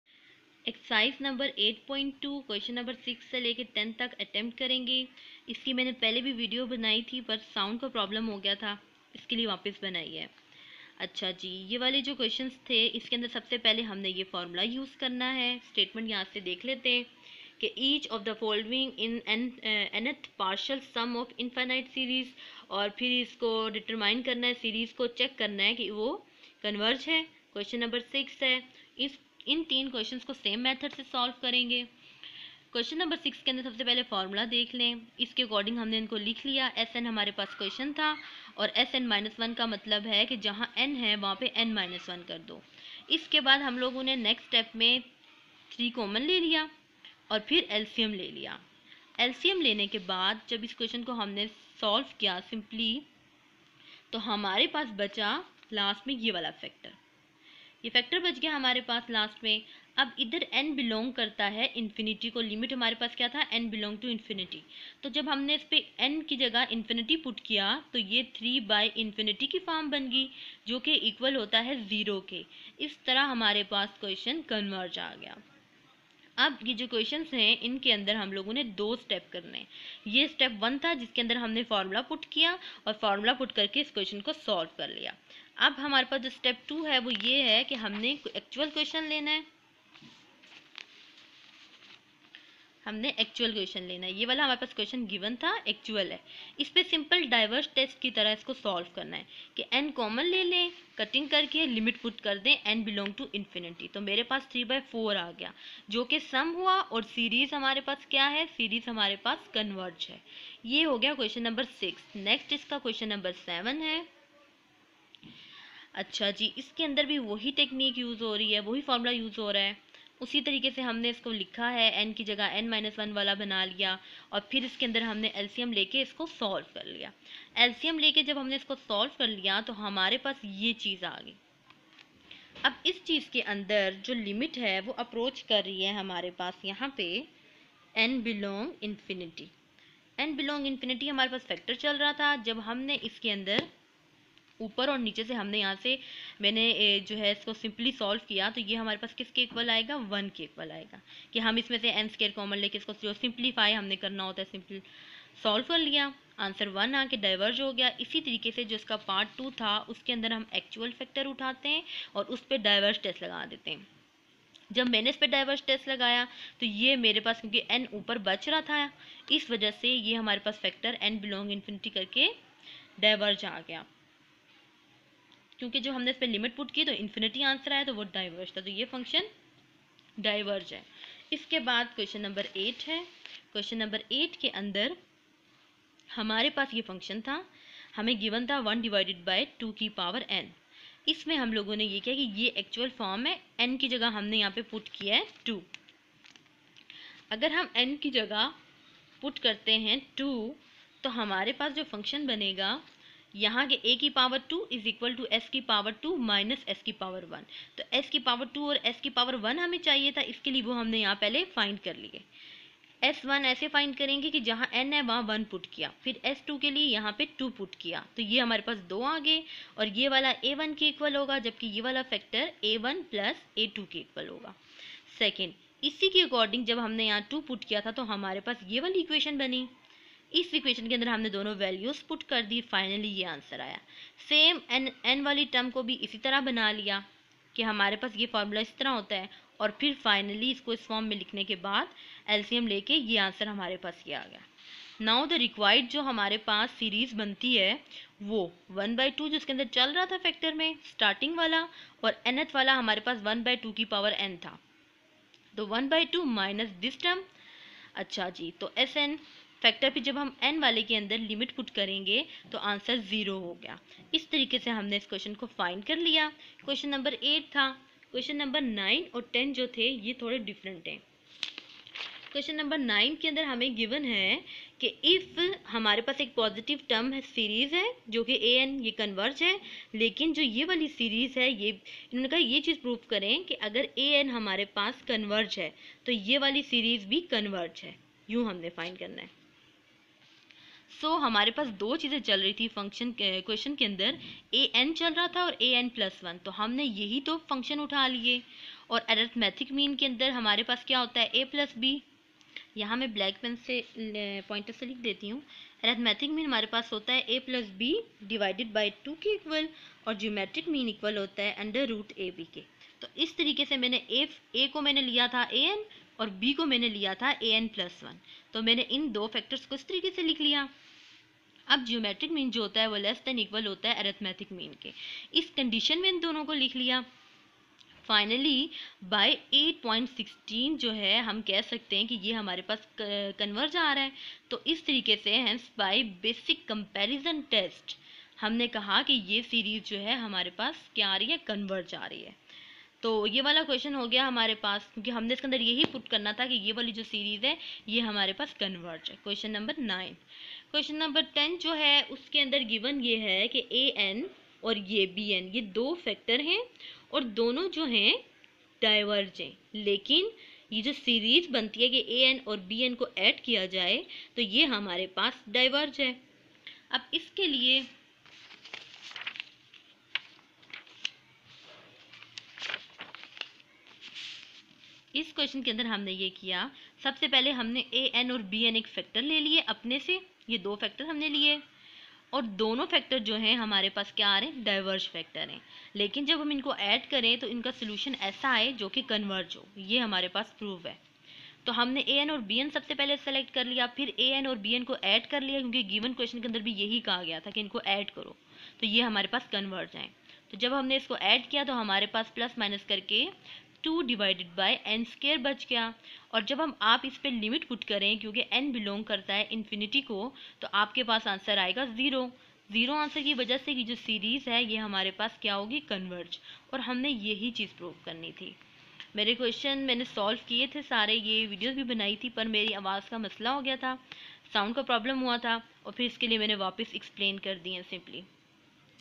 Exercise 8.2/Part2/ Mathematical Methods by SM YUSUF.
AOA;
Each of the following is the nth partial sums of an infinite series.Determine the series and check whether it converges.
solving question no.(6 to 20).